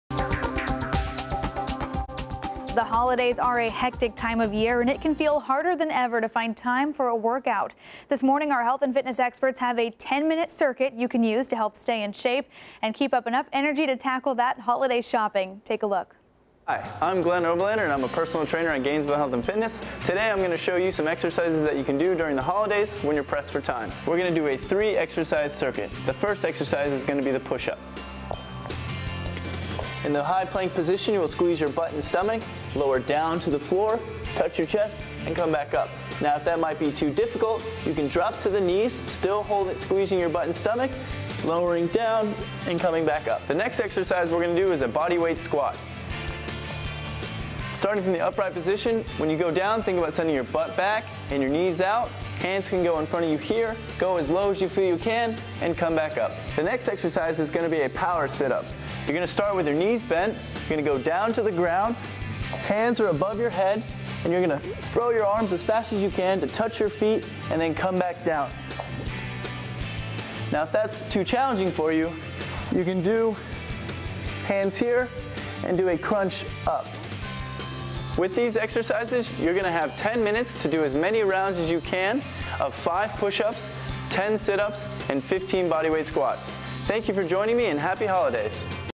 The 10 Minute Healthy Holiday Workout
GHF Personal Trainer Glenn Oberlander takes you through a great 10 minute workout perfect for the holidays or any day!